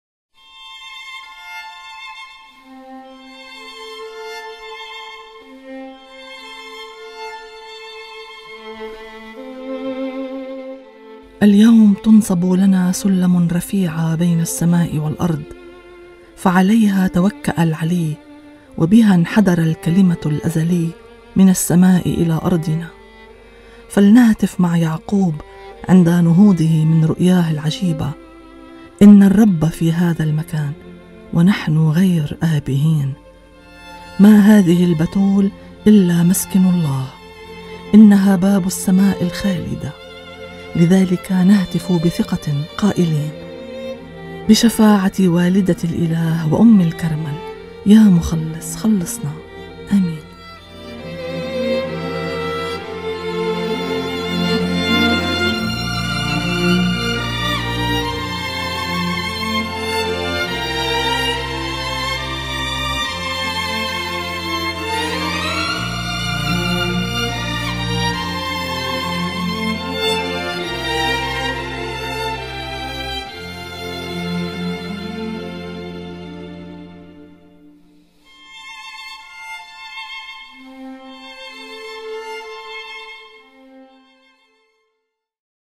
صلاة تساعيَّة الطوباويَّة مريم العذراء، والدة الإله، سيّدة جبل الكرمَل - اليوم السادس - 12 تمّوز
كتابة | الراهبات الكرمَليَّات الحافيات (دير كرمَل والدة الإله والوحدة - حريصا)
صوت | منى عجاقة
تسجيل | استوديو إذاعة صوت المحبّة
إشراف | الأب نوهرا صفير الكرمَلي
تنفيذ | نور الحاج
إنتاج | رهبانيَّة الكرمَليِّين الحفاة في لبنان
© Ordre des Carmes Déchaux au Liban